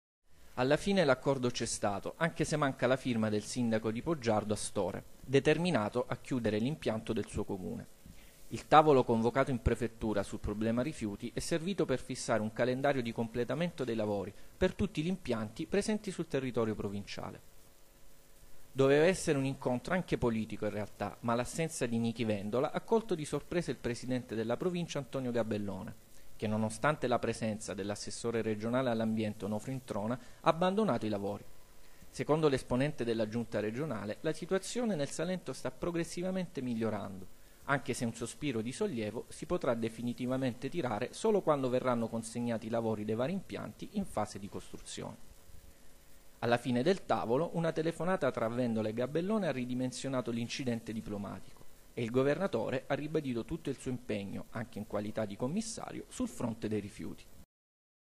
RIFIUTI: VERTICE IN PREFETTURA
Senza il commissario Vendola si è svolto in Prefettura a Lecce un vertice sui rifiuti. Definito, tra qualche polemica, un calendario per il completamento dei lavori relativi ai vari impianti. La situazione, al momento, appare decisamente migliorata rispetto a qualche settimana fa.